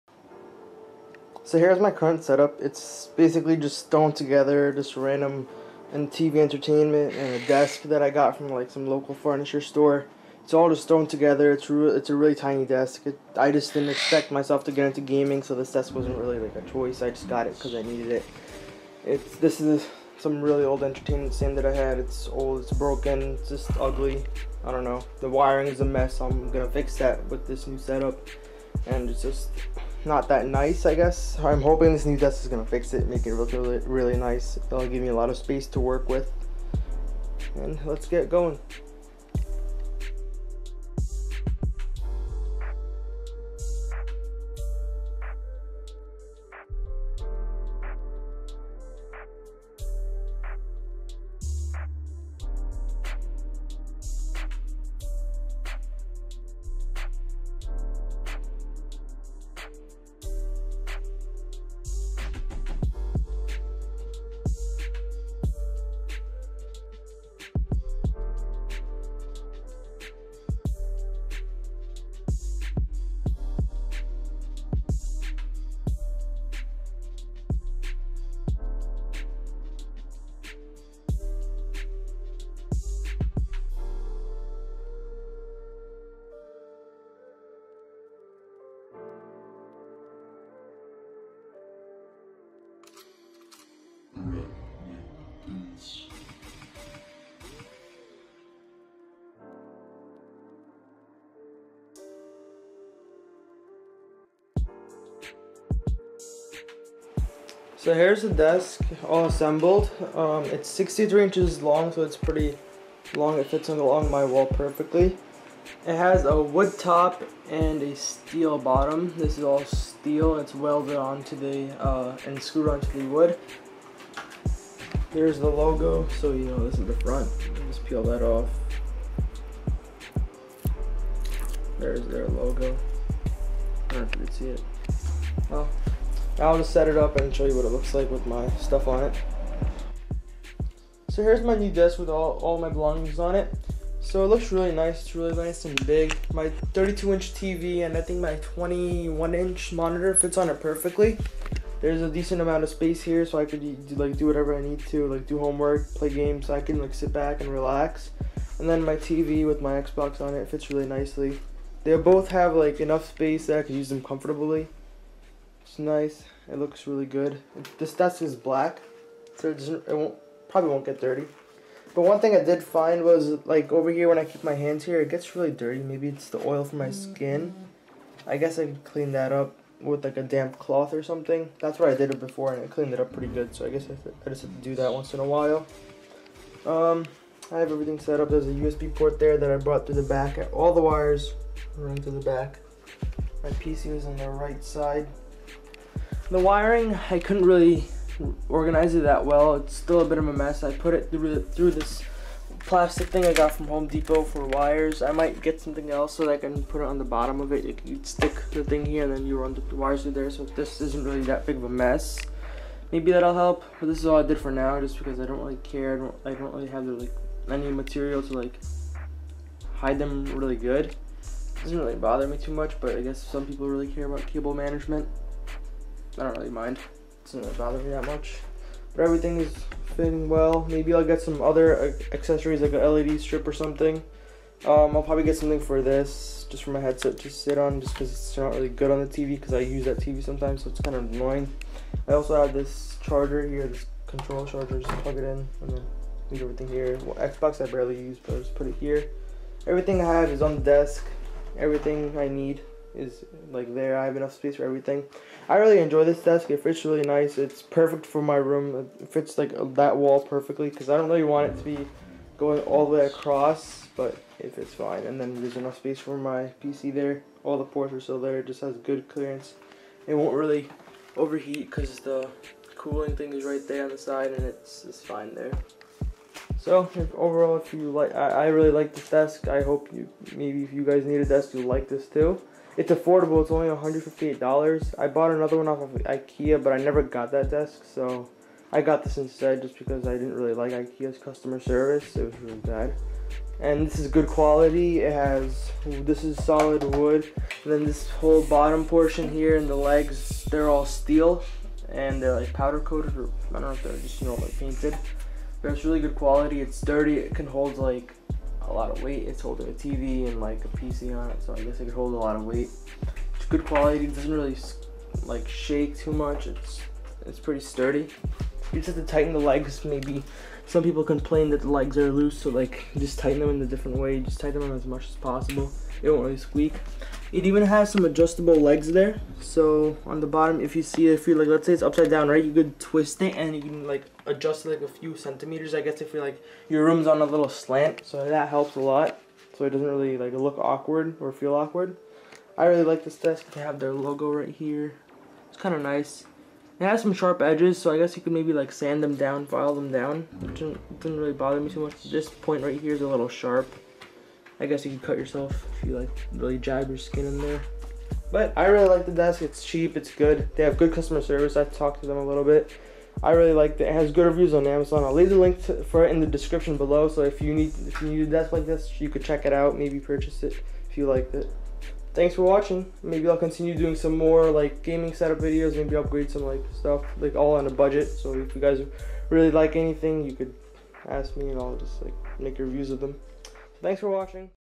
BEST Budget Gaming Desk | Need 63" Computer Desk
Also a very popular option by my viewers

I will be reviewing the Need 63" Desk I purchased for my gaming setup. It is very nicely made, and is perfect for my use. I would highly suggest this desk if you are in need of a good quality desk for your setup.

★ Amazon Links ★

• Gigabyte GTX 1050ti
• Thrustmaster TMX
• Thrustmaster TH8A
• Thrustmaster T3PA
• TMX + T3PA Bundle
• T3PA Pro
• HyperX Cloud Stinger